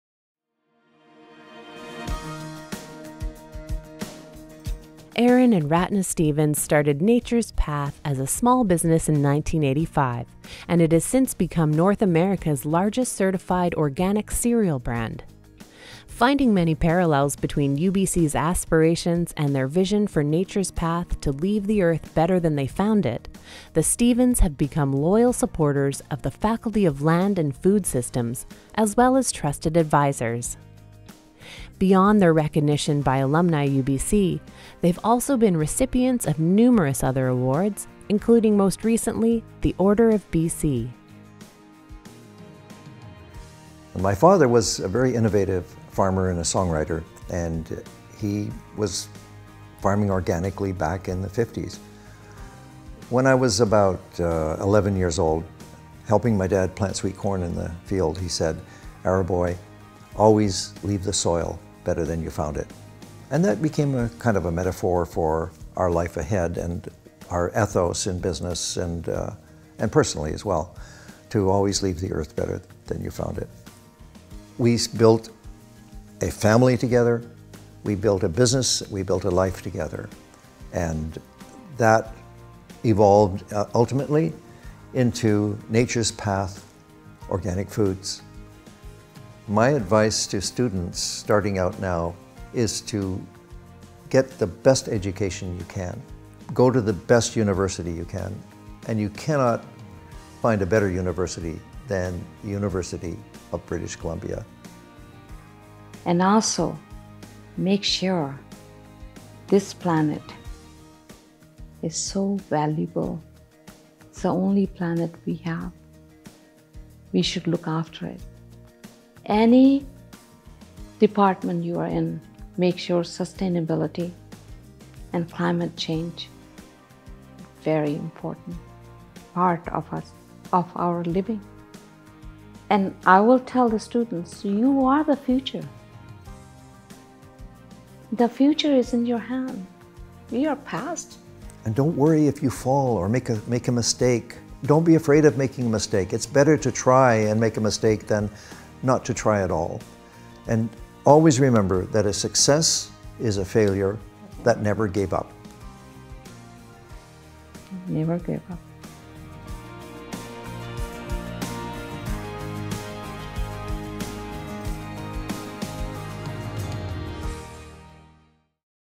Arran and Ratana Stephens, OBC – alumni UBC 2021 Achievement Awards
Honorary Alumni Award - Arran and Ratana Stephens started Nature’s Path as a small business in 1985, and it has since become North America’s largest certified organic cereal brand. Finding many parallels between UBC’s aspirations and their vision for Nature’s Path to leave the earth better than they found it, the Stephens have become loyal supporters of the Faculty of Land and Food Systems, as well as trusted advisors. Beyond their recognition by alumni UBC, they’ve also been the recipients of numerous other awards, including most recently, the Order of BC.